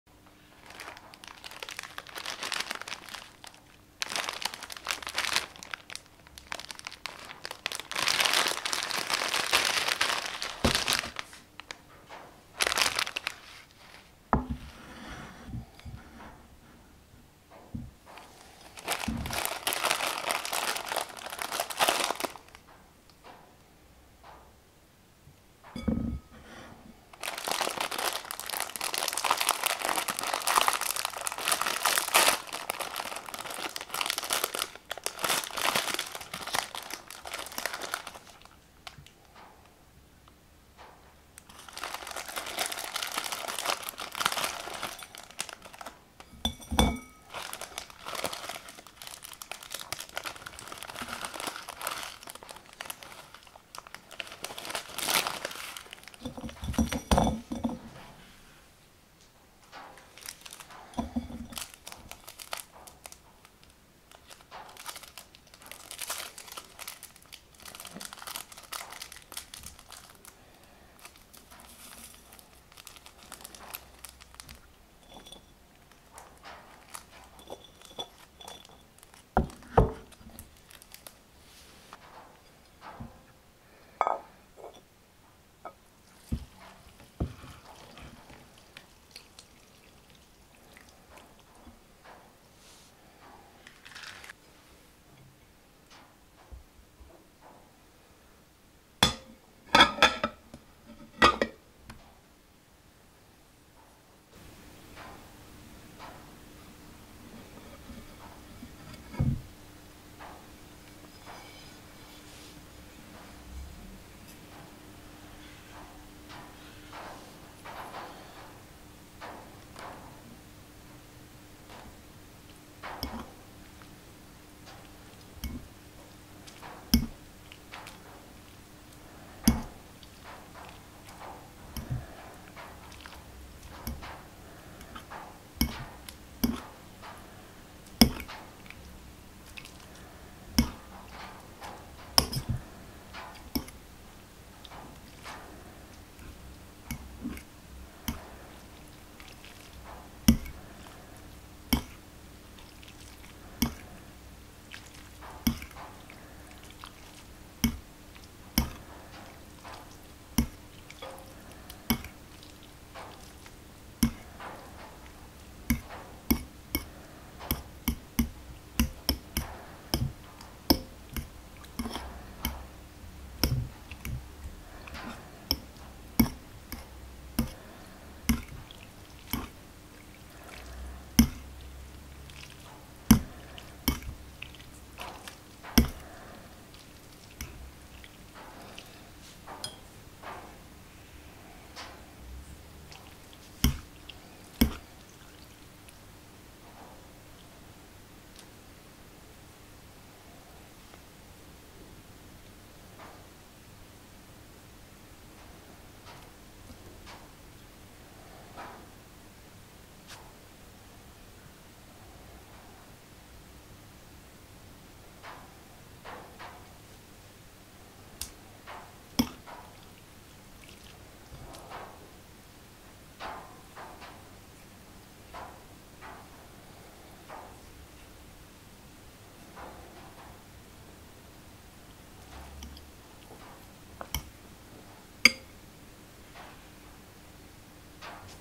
Noodles From Eastern Europe | Fast Food | Preparing Noodles| Restart Kitchen | ASMR | Maggi Noodles
Hello food lovers! Today I show you Maggi Noodles with vegetables. Cheap and easy to prepare. Restart Kitchen is a channel about unboxing, cutting and preparation of food. Enjoy!

Have a video Suggestion? Post it in the Comments Section